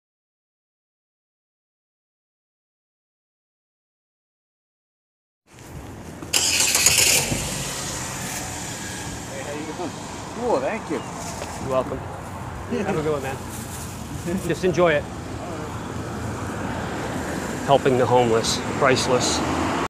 See someone down..give them a boost
Helping the homeless whenever I can or anyone else in need